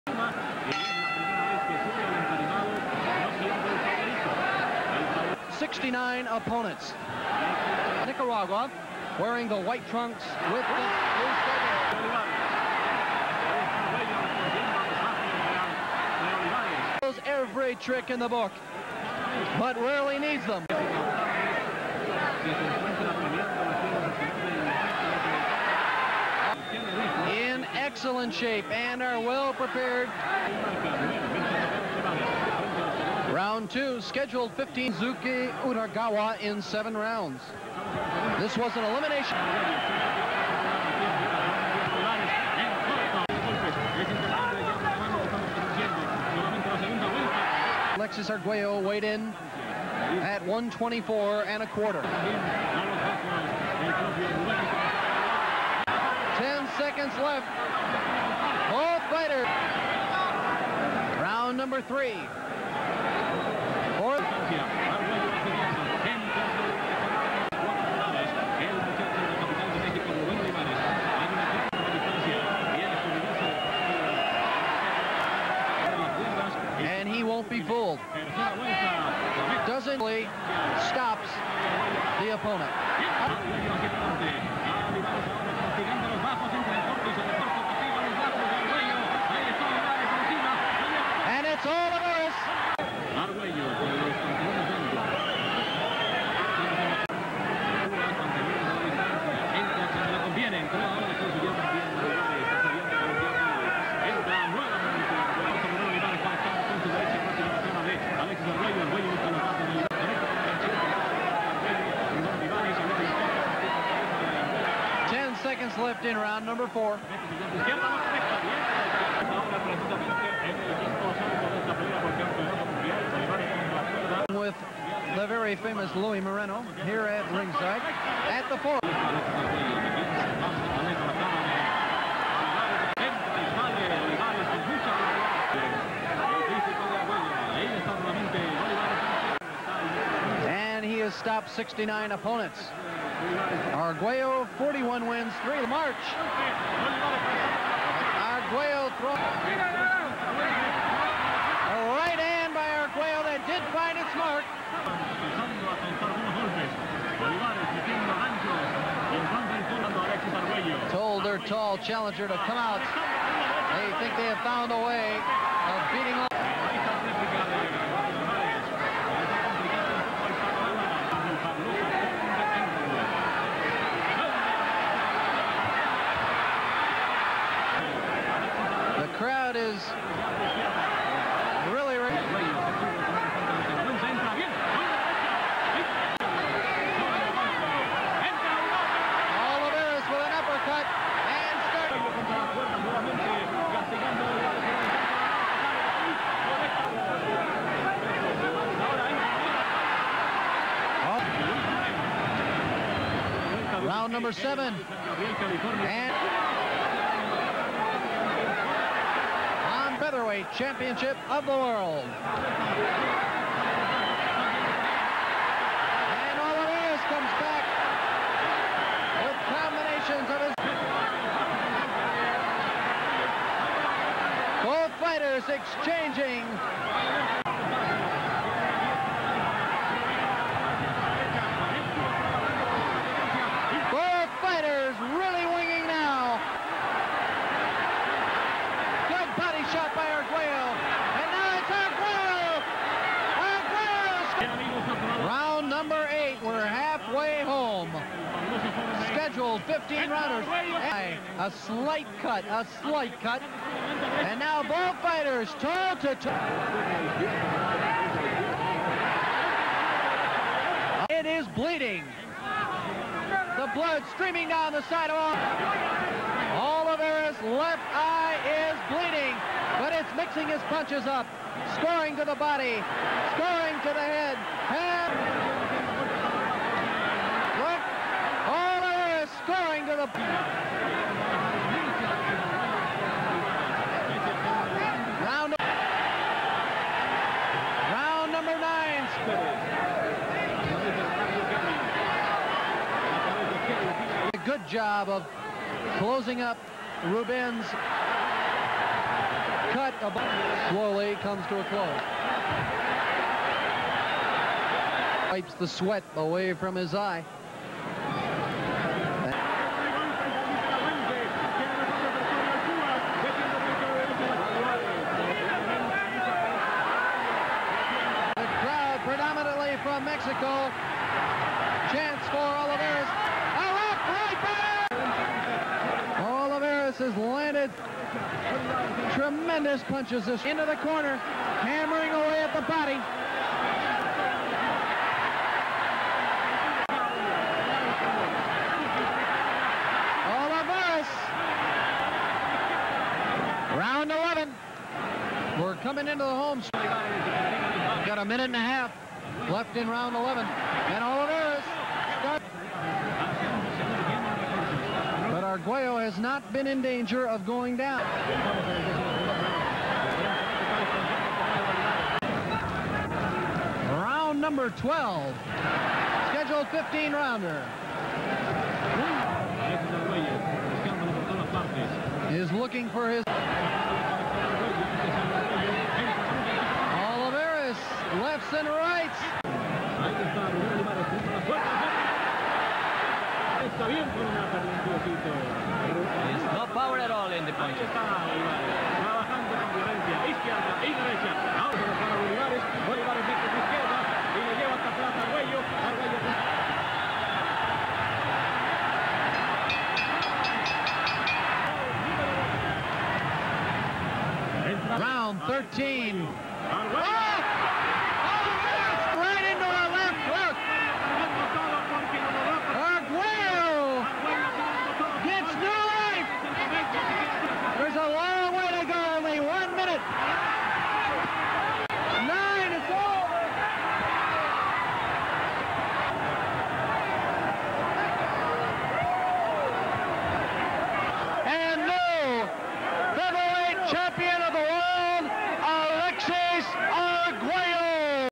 Alexis Arguello Vs Ruben Olivares Highlights (WBA Title)
"El Caballero del RING" Alexis Arguello Vs Rube "Puas" Olivares. Por el Titulo Mundial WBA de Peso Pluma. Olivares busca la 3era. Defensa de su Titulo.